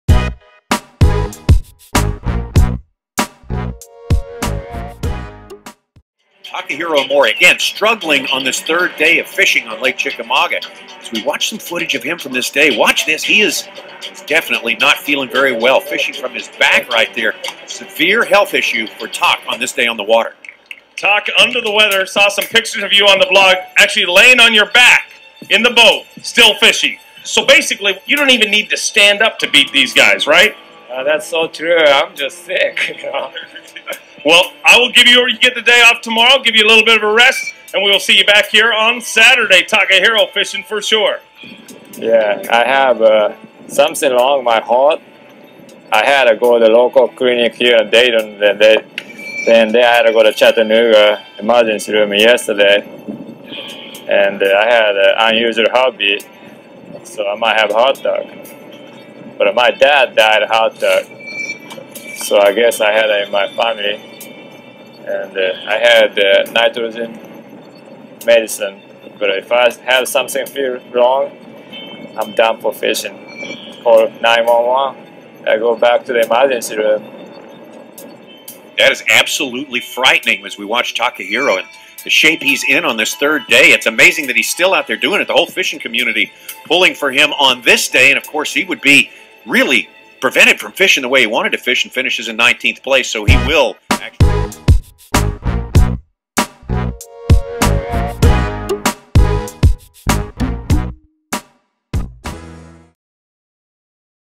Takahiro Omori Has Near Heart Attack On The Water BassFest
Takahiro Omori fishes from his back and carries nitorgen medicine as he almost has a heart attack on the water during BASSFest.

These guys are tough.

Credit: ESPN2, Bassmaster.com, Bassmaster